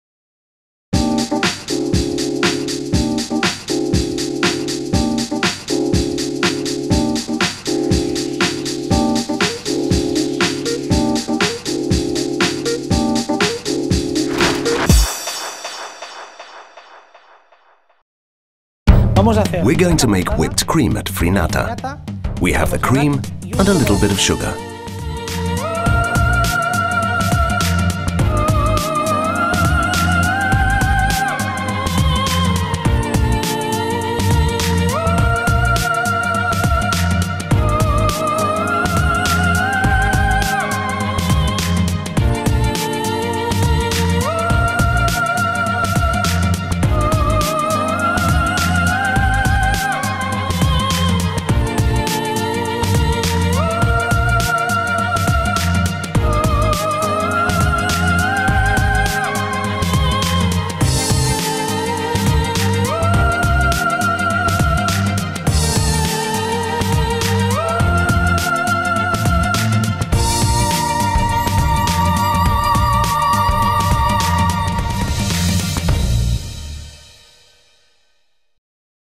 Finata refrigerated cream whipper (aerating mixing)
The refrigerated cream whipper FRINATA achieves owing to its double cooling system giving to the cream a really white colour and extending its stability. This makes it possible to every pastry customizing its cream by controling the 5 parameters of the whipping:

The use of cold air during the whipping between 2/4ºC isolates the cream from the heating of the mixing (patented system).
Adjust the quantity of air insufflated.
Adjust the speed of agitation.
Allows to set the whipping time cycle.
The cream is maintained at the bucket between 2/4ºC during the whole working day.

Frinata is the only horizontal axis cream whipper in market, this way we get an homogeneous whipped cream, mousse, bavaroise, swiss cream, etc..